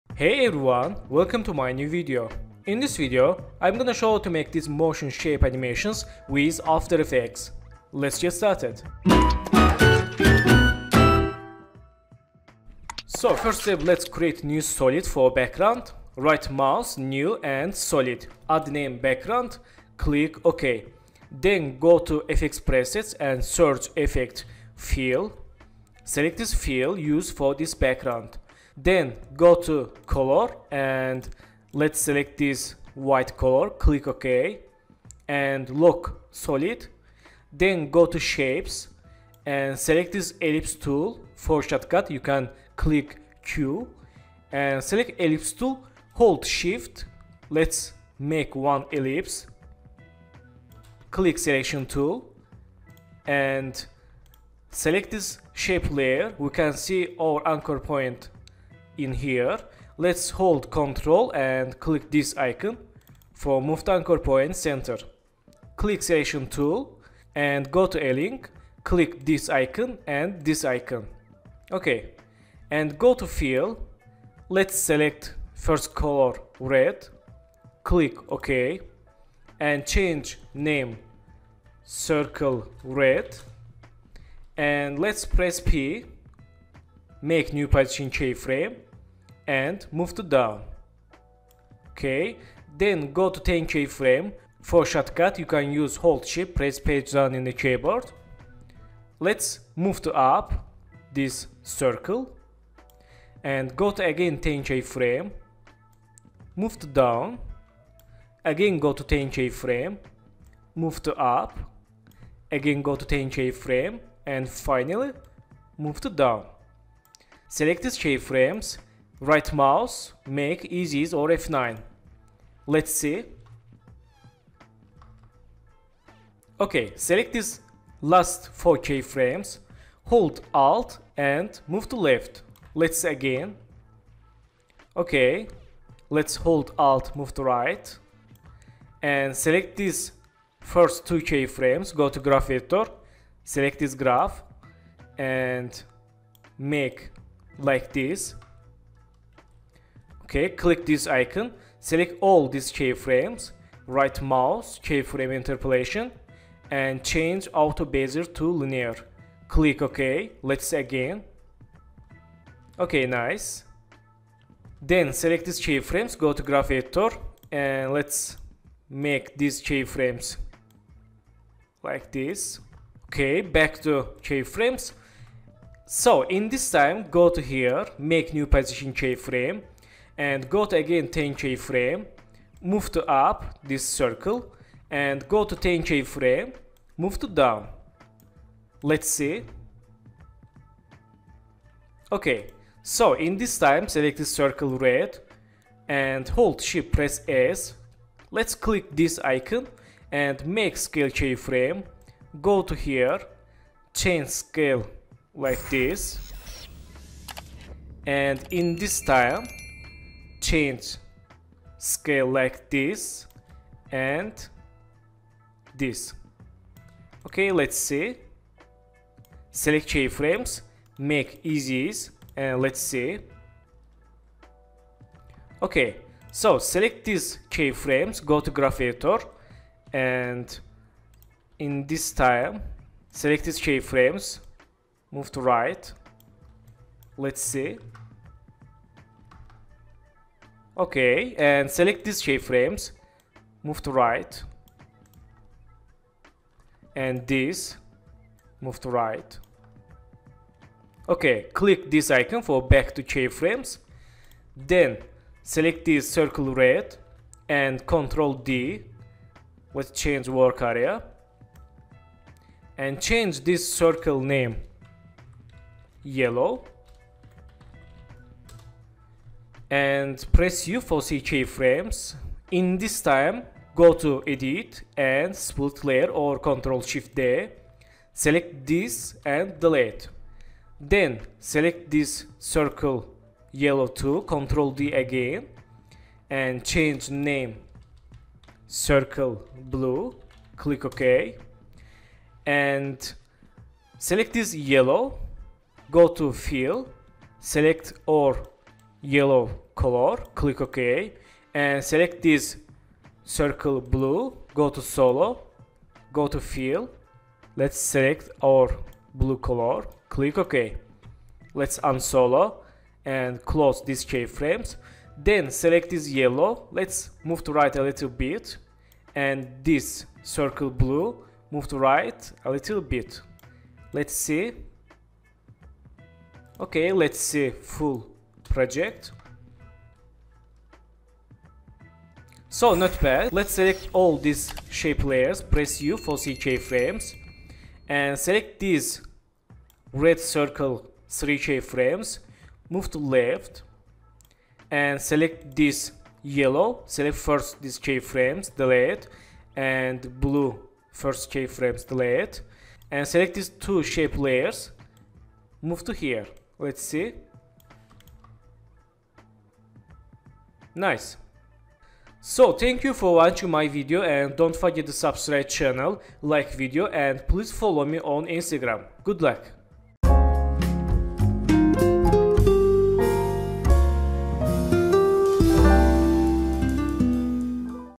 Motion Design With Shape Layers in After Effects Tutorial
Hi guys, welcome to my new After Effects tutorial. In this tutorial i will show how to create motion design animations with only shape layers. This is very clean and easy. Let's get started!